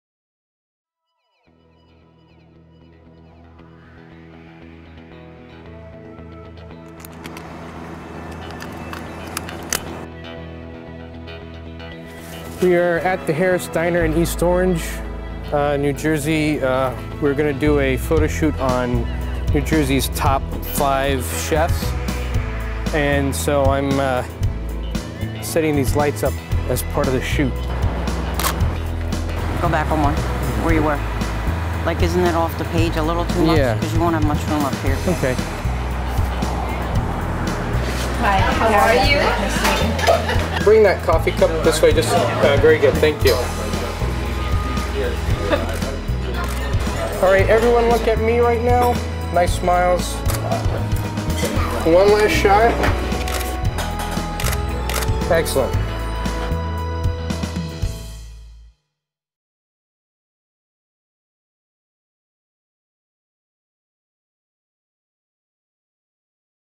Behind the scenes at an Inside Jersey magazine photo shoot
Travel behind the scenes of the top chefs of New Jersey photo shoot for Inside Jersey magazine. (Video by John O'Boyle/The Star-Ledger)